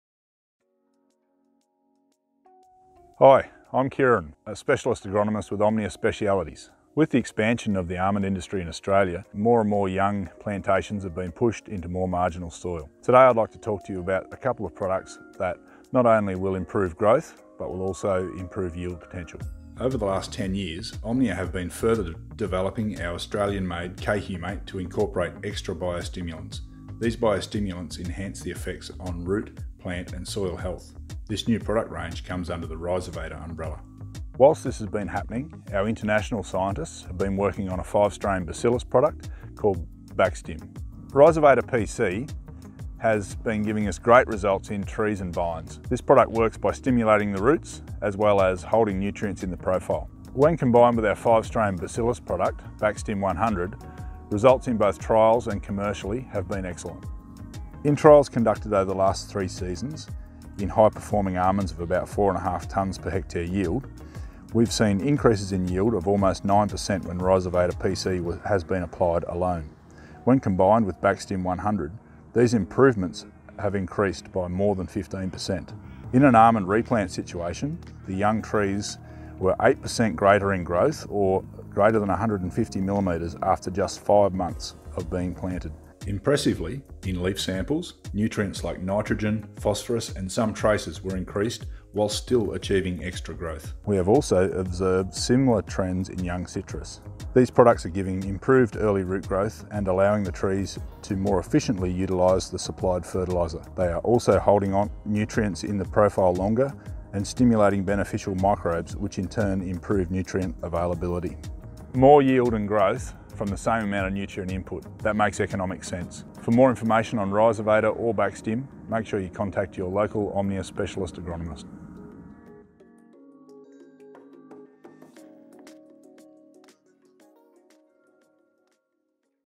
Improving growth and yield potential of almonds by leveraging biological synergy
Kieran describes how to improve growth and yield potential in almonds and young citrus by harnessing the biological synergy of two Omnia products, Rhizovator and Bacstim 100.  This combination acts as a biostimulant by encouraging root growth, introducing beneficial bacillus species and holding nutrients in the root zone longer leading to improved nutrient uptake and plant health.
Omnia trials have recorded a 15% increase in almond yields compared to the growers normal practice.
In young trees trials have shown an increase in nutrient uptake resulting in increased canopy size and trunk diameter.
Rhizovator and Bacstim from Omnia may increase yields and overall plant health resulting in a better bottom line for the grower. For more information on Rhizovator and Bacstim please

To contact your nearest Omnia Specialist Agronomist